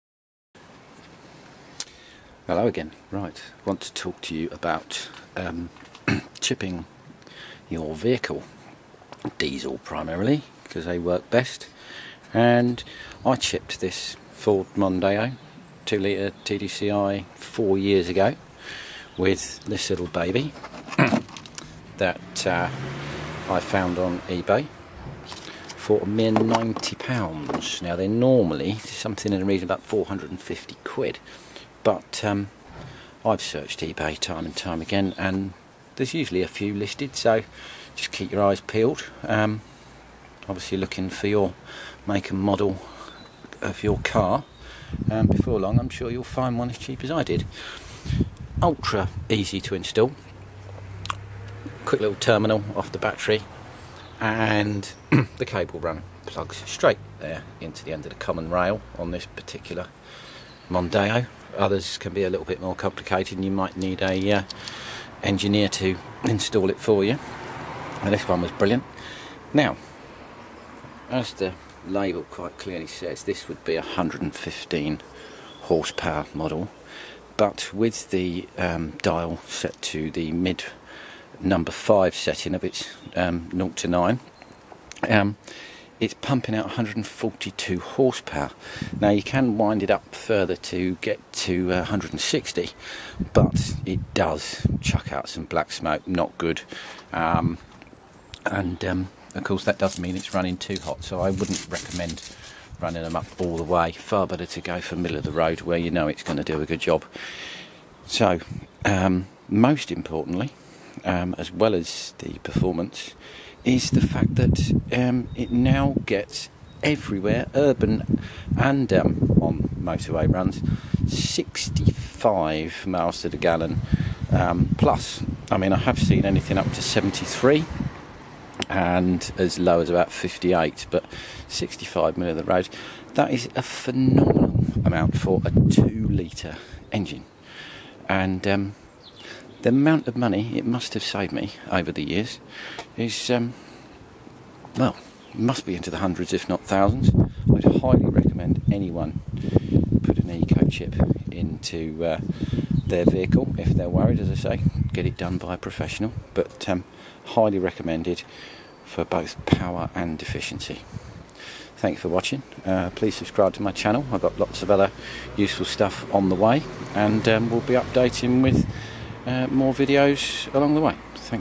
Mondeo 2.0l TDCI Eco Performance Remap Chip Install 65+mpg 160hp
Mondeo 2.0l TDCI Mk3
Eco Performance Remap Chip Install
65+mpg 160hp

I installed my chip over 4 years ago and am so glad i did!
It has saved me many hundreds if not thousands of pounds in fuel, plus made the car so torquey too, much more fun to drive too !

Installing the chip took 5 minutes, by just connecting the terminal to the battery and plugging in the loom to the common rail

I only paid £90 for the chip from ebay, worth every penny